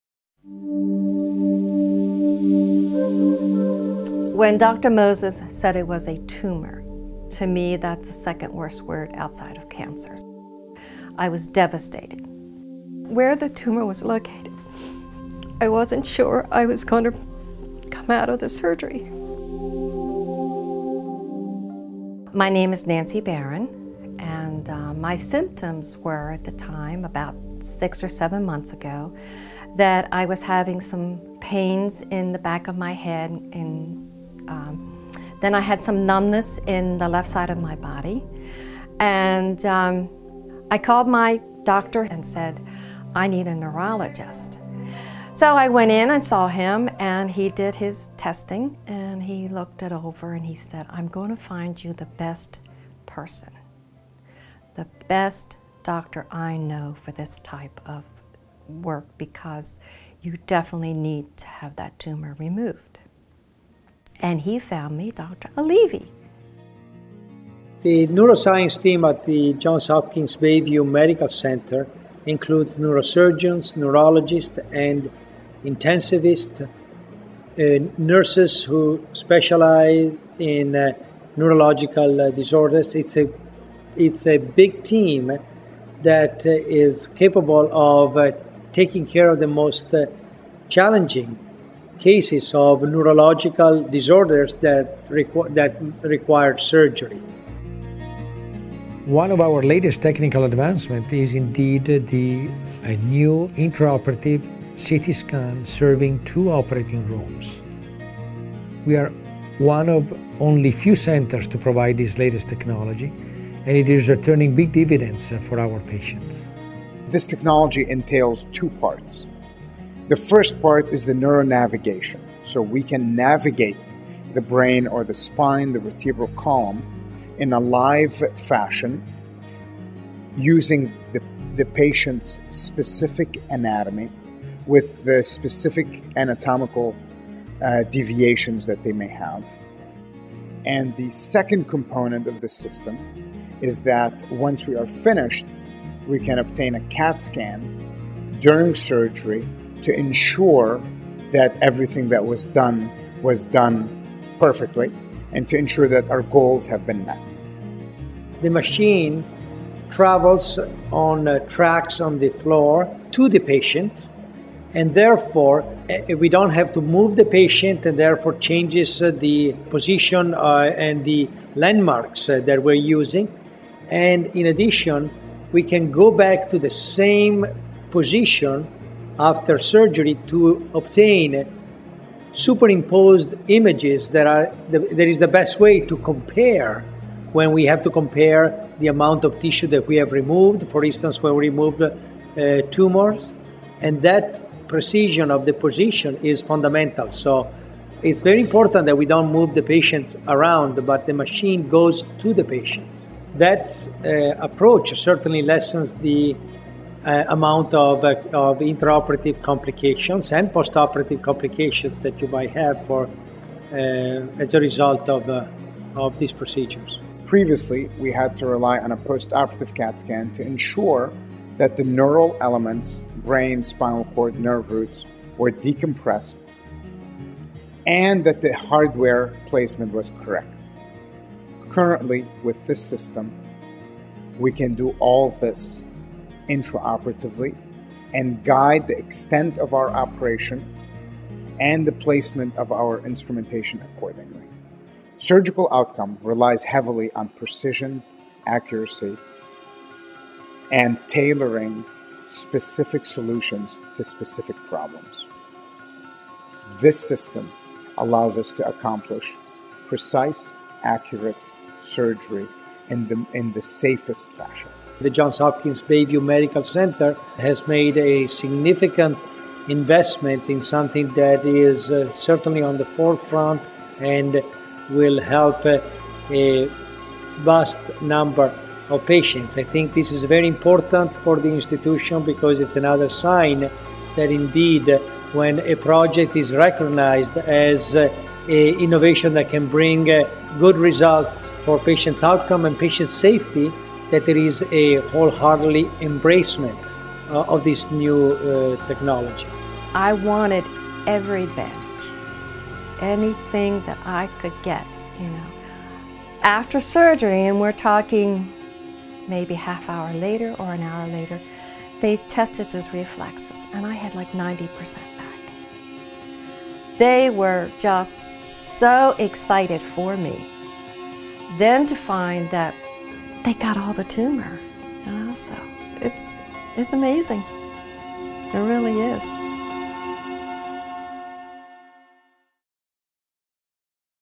New Dual iCT at Hopkins Bayview Medical Center
Johns Hopkins Bayview Medical Center is the first hospital in Maryland with a dual-room intraoperative CT scanner and image-guided surgery system for neurosurgery. Having imaging capabilities in two of our operating rooms has a positive effect on our patient outcomes by improving safety, decreasing infections and lowering the risks of complications. The intraoperative CT (iCT) and image-guided surgery system create unmatched surgical vision and precision, allowing surgeons to see things others cant see during complex brain, spine and trauma surgeries.